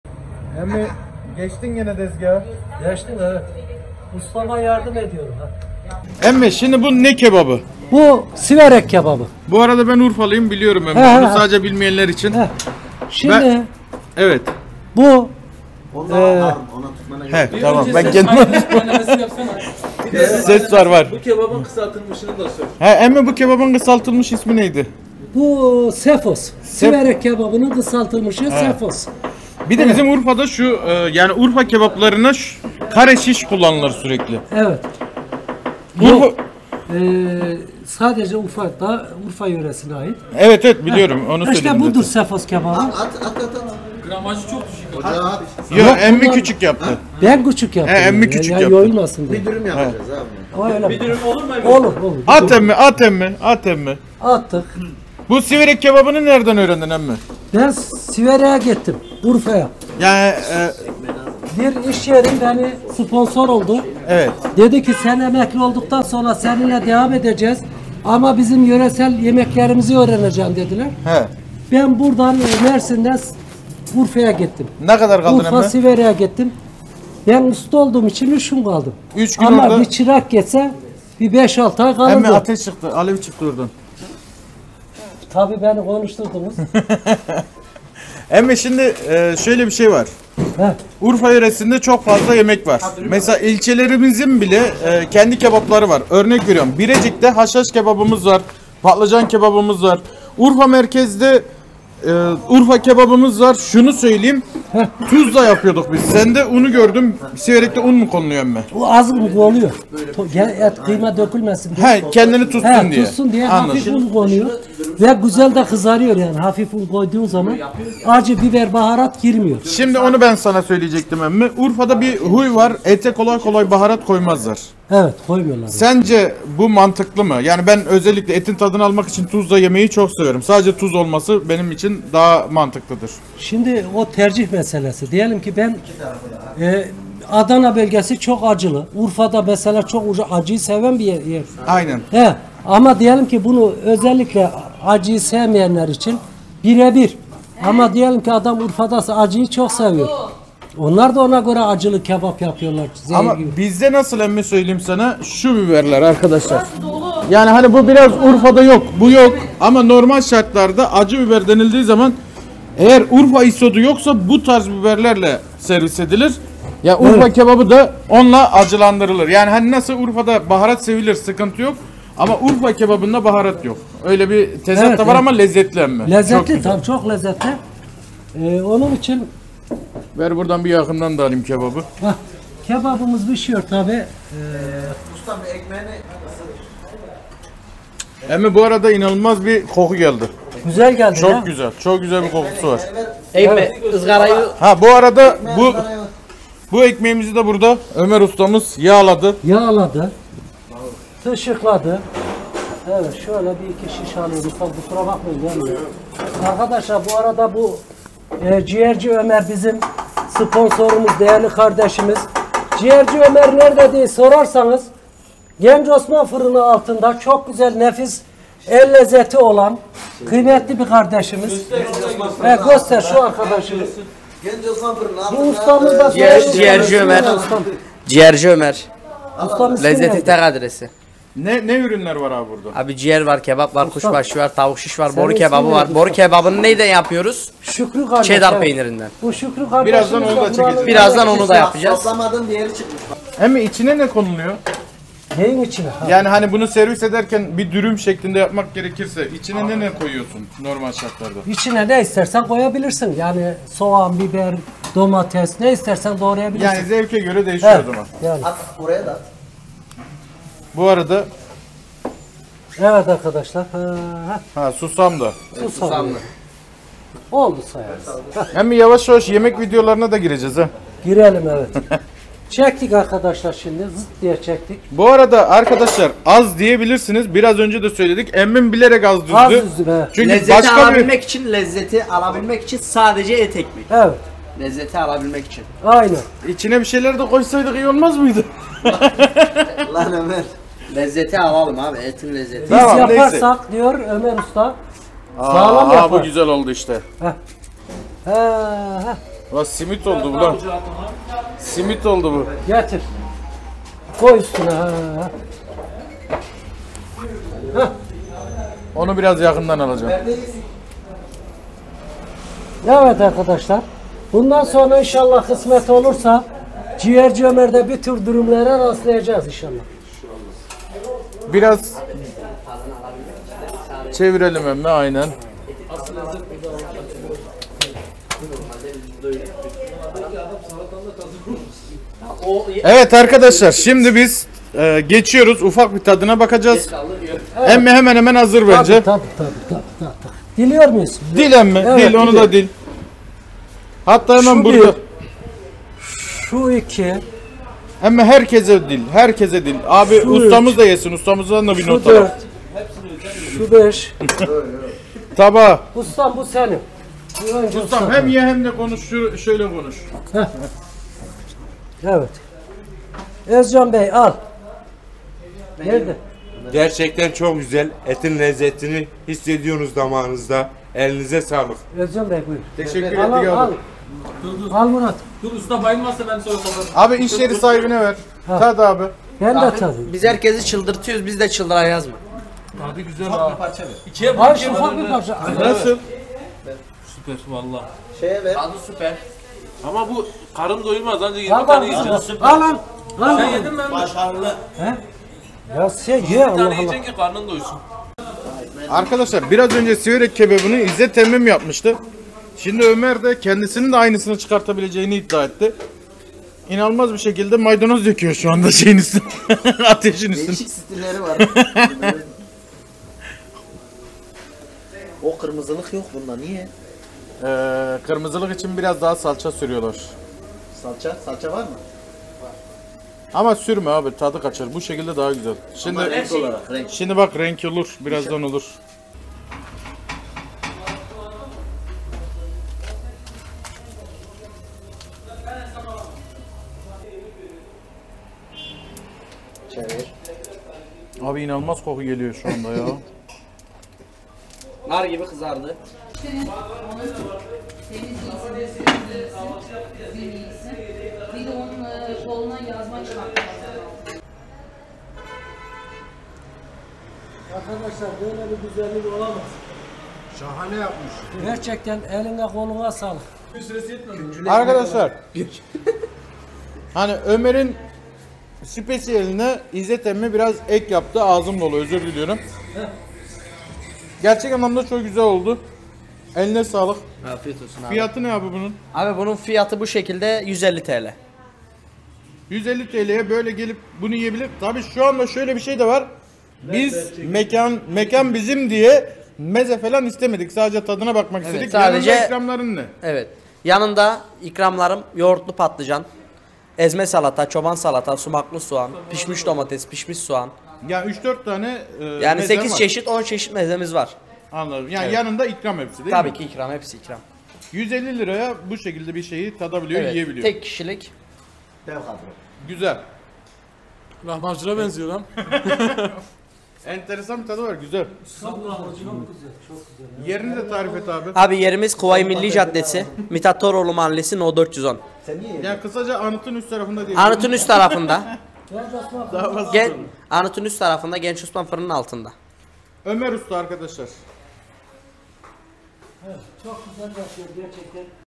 İzzet Emmi'den Siverek Kebabı - Ömer Usta
Herkese merhabalar arkadaşlar İzzet Emmi Ciğerci Ömer Usta'da Ocakbaşına geçti ve Şanlıurfa yöresine ait Siverek Kebabı yaptı iyi seyirler...